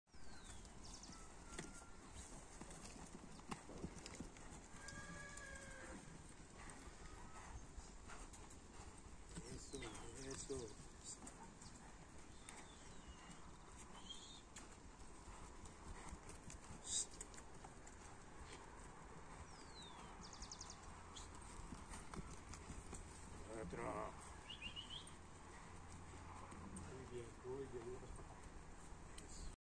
Potro Paso Fino Colombiano a la cuerda
Don Martin potro de 18 meses a la cuerda, mostrando su potencial en el paso fino Colombiano